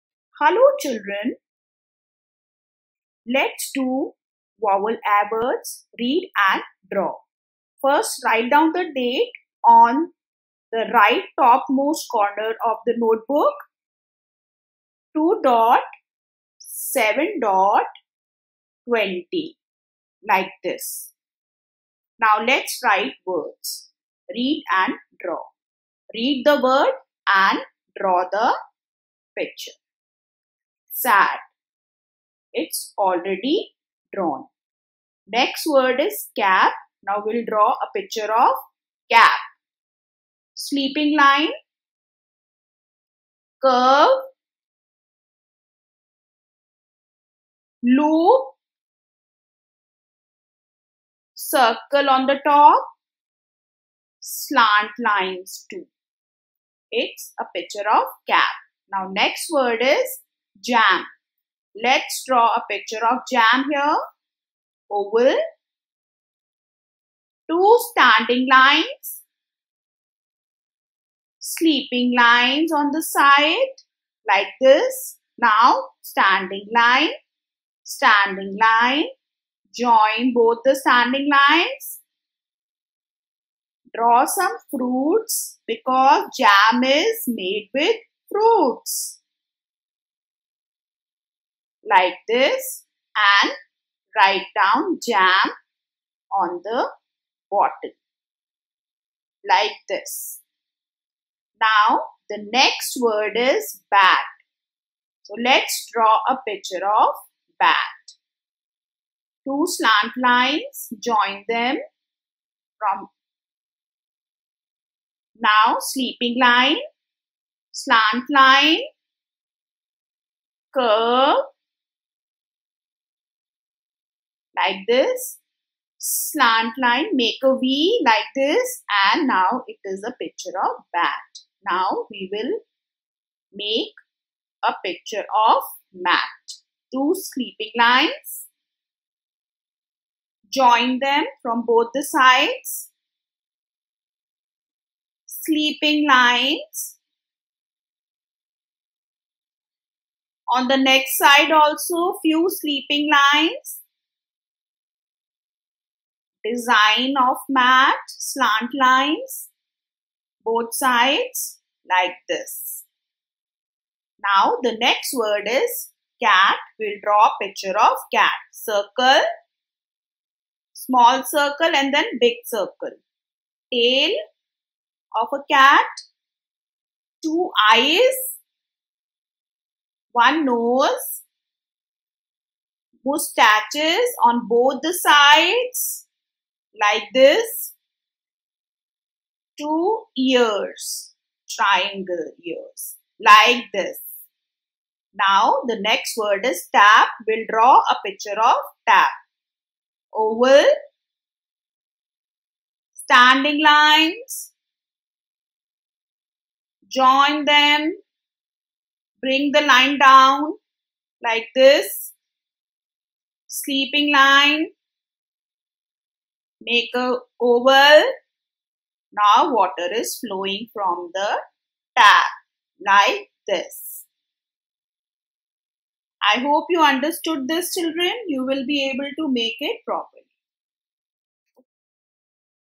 Read and Draw
Tutorial on reading and drawing the three letter words pictures prepared by the facilitator of MLK DAV DWARKA